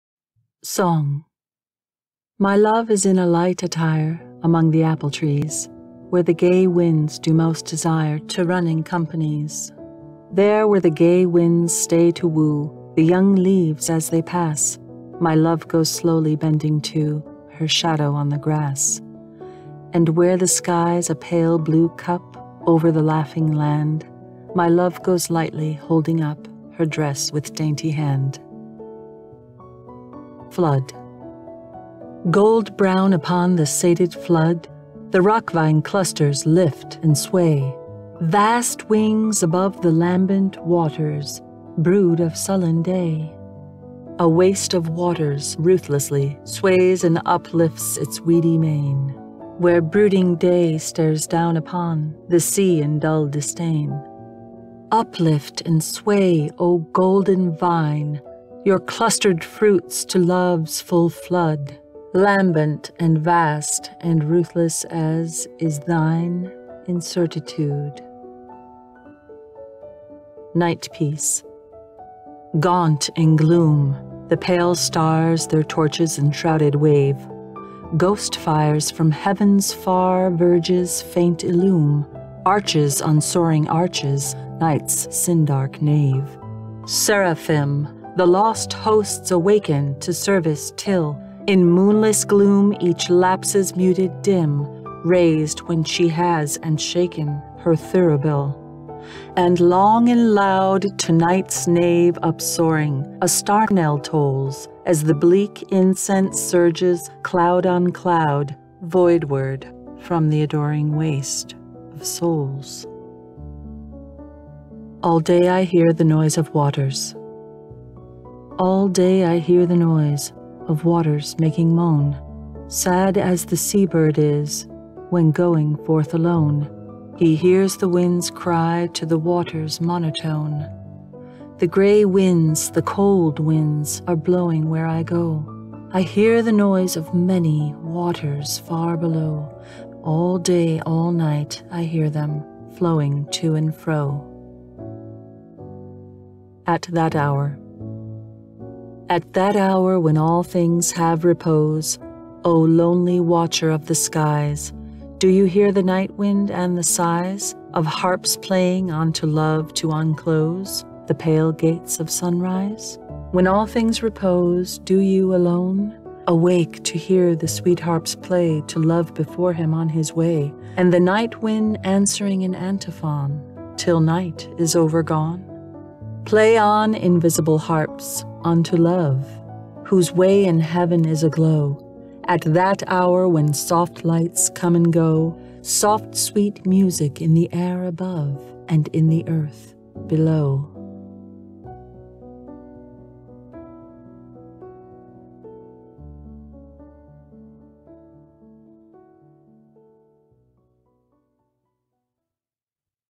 James Joyce - Song - Flood - Night Piece - All Day I Hear The Noise Of Waters - At That Hour
Relaxing read of some of James Joyce's most beloved poems by the narrator Amy Ann Weideman.